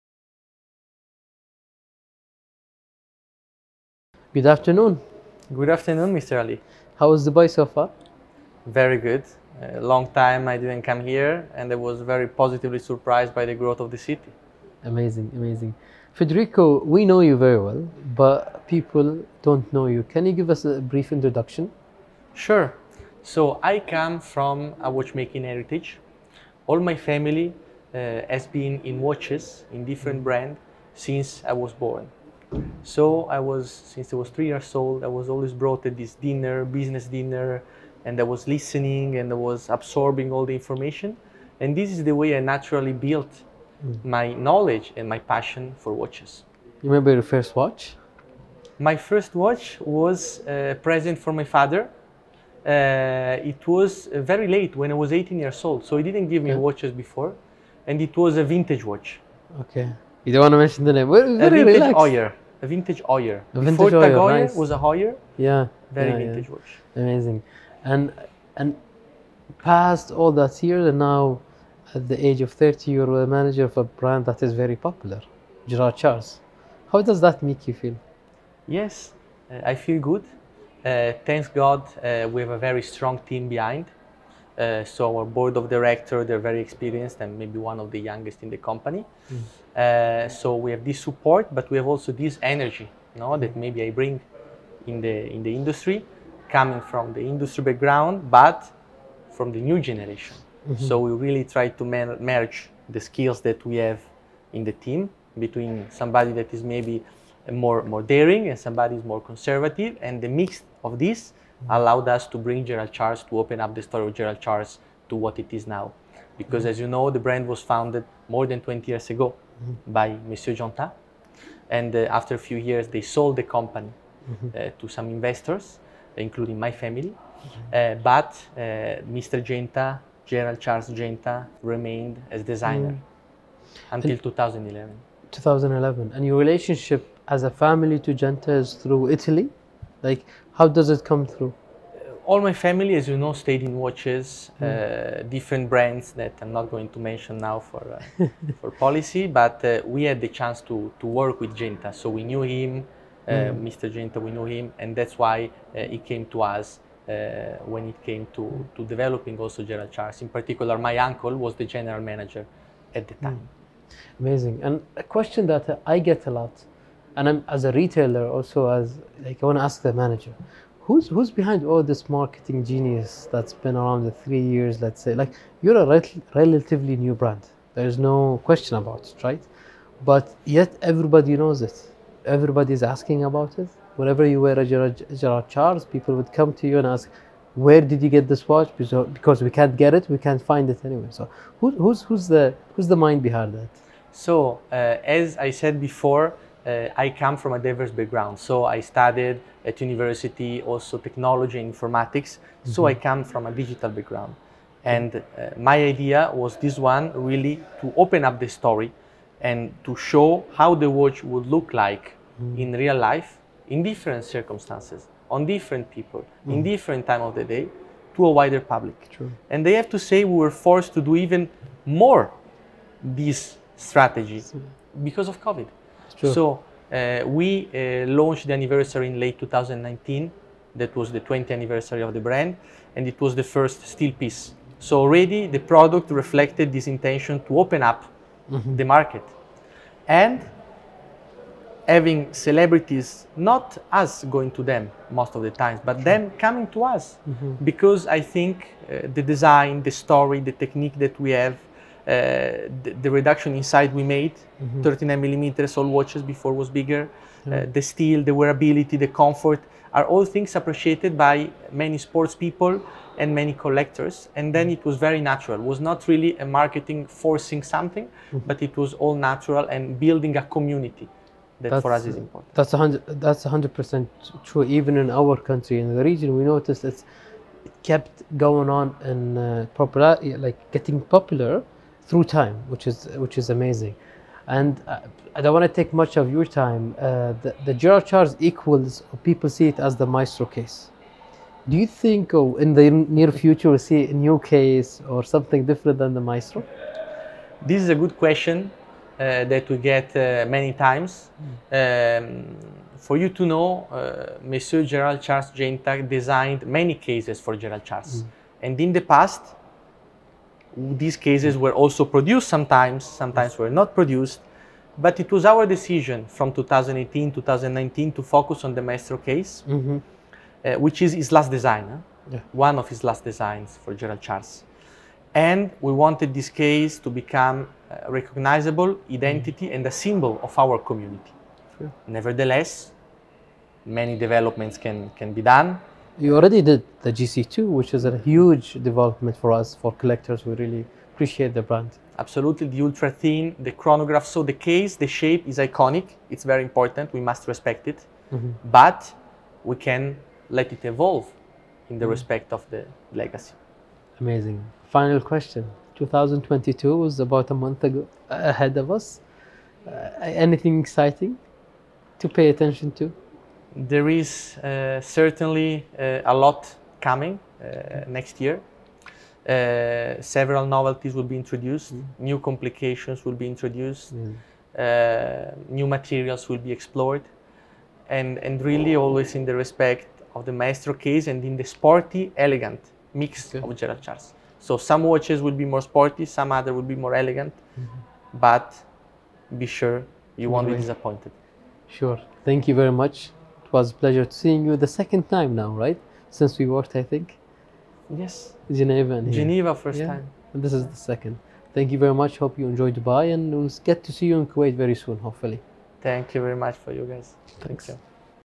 فيدريكو زيفياني من شركة جيرالد تشارلز | اسبوع دبي للساعات ٢٠٢١
مقابلة مع مالك والرئيس التنفيذي لشركة جيرالد تشارلز
فيدريكو زيفياني

تايم كيبر الموزع الرسمي للشركة في الكويت، لتفاصيل أكثر يمكنكم التواصل معنا